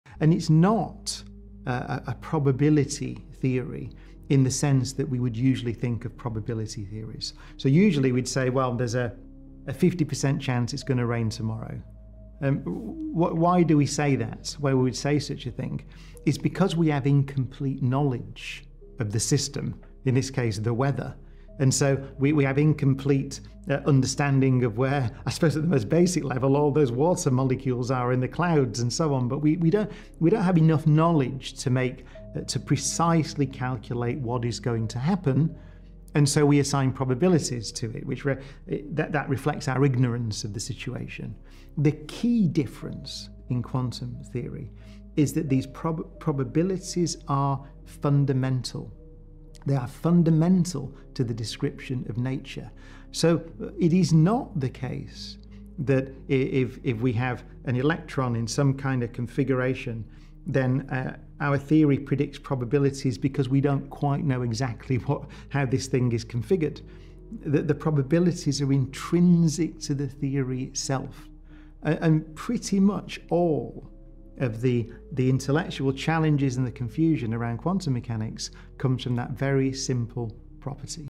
Quantum vs Classical: Why Probability is SO Different!
Quantum theory's probabilities aren't due to incomplete knowledge, but are fundamental to nature itself. This intrinsic uncertainty is where much of the confusion arises.